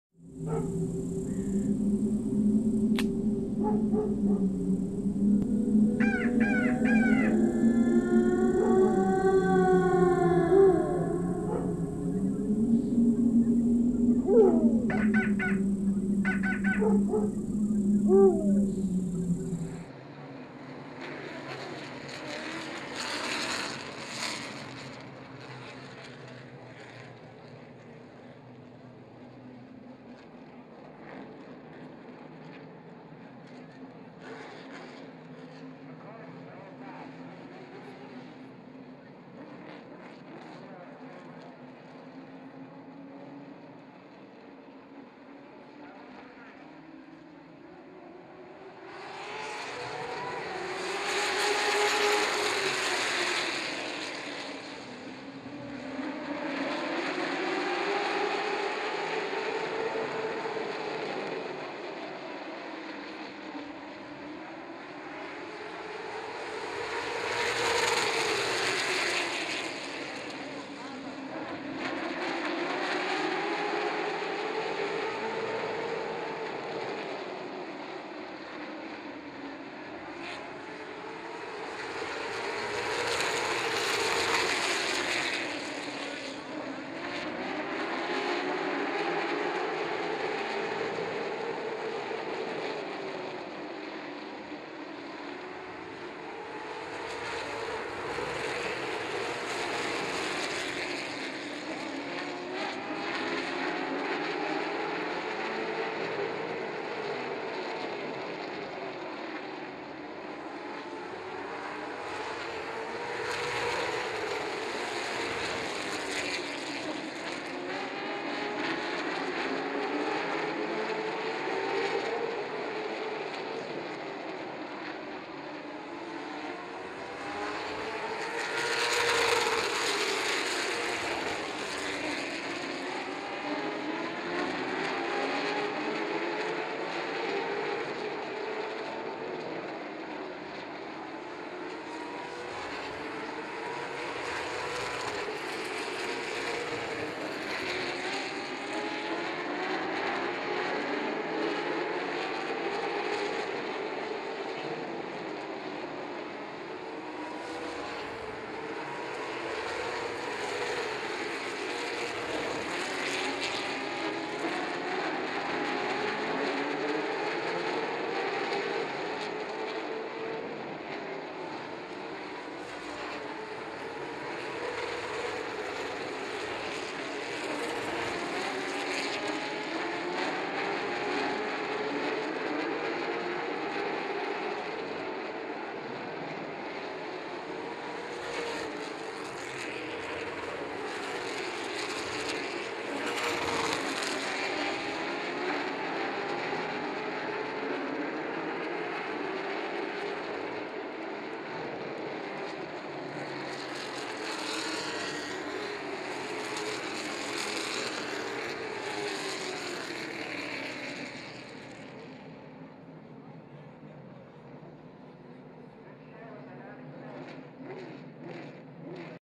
Full LMSC Heat 2 , Oct. 31 , 2015
Fright Night 300 , Oct. 31 , 2015
Heat Race #2
LMSC 125 Laps $6,000 to Win
Street Stocks 30 Laps $1,000 to Win
Pure 4 40 Laps $500 to Win

Video / Graphics / owned by Richard Ford